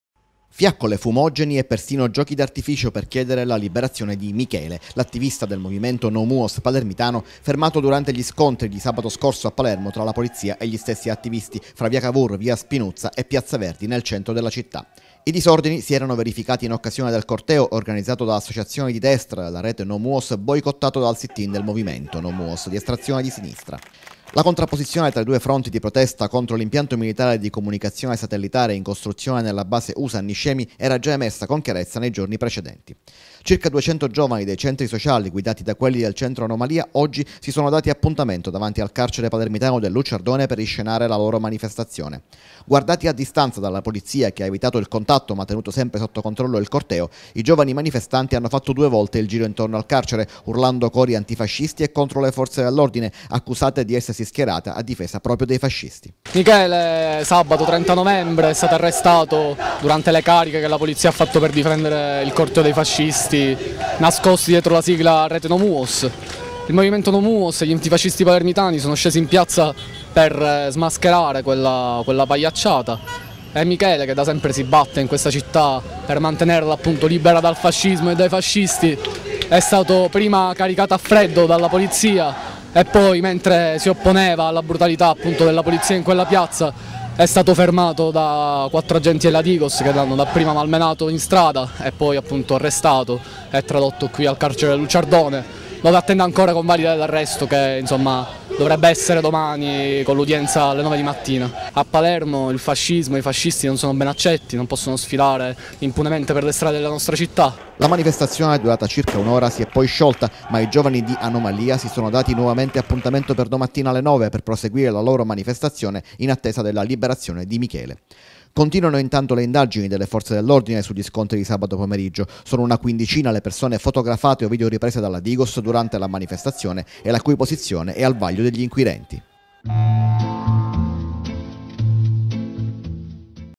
Palermo, i No Muos ''assediano'' l'Ucciardone
Fiaccole, fumogeni e perfino giochi d'artificio per chiedere la liberazione di Michele, l'attivista del movimento No Muos palermitano fermato durante gli scontri di sabato a Palermo   fra via Cavour, via Spinuzza e Piazza Verdi nel centro di Palermo. I disordini si erano verificati in occasione del corteo organizzato associazioni di destra della Rete No Muos, boicottato dal sit-in del Movimento No Muos, di estrazione di sinistra.  La contrapposizione tra i due fronti di protesta contro l'impianto militare di comunicazione satellitare in costruzione nella base Usa a Niscemi era già emersa con chiarezza nei giorni precedenti.
Servizio di  Manlio Viola
Editing  di  Marcella Chirchio - Claudia Autore